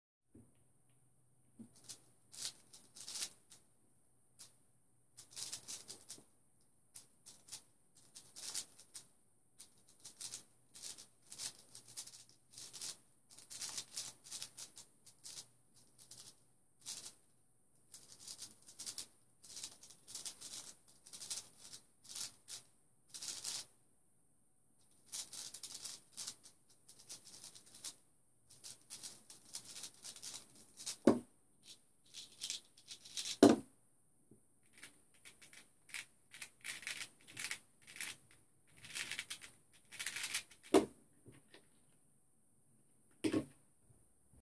1:18.542 2x2-4x4 Relay! (2x Speed)
This was a decent relay, but I wanna get a sub 1:15 on cam.

Cubes:
2x2: Dayan
3x3: Tanglong
4x4: Aosu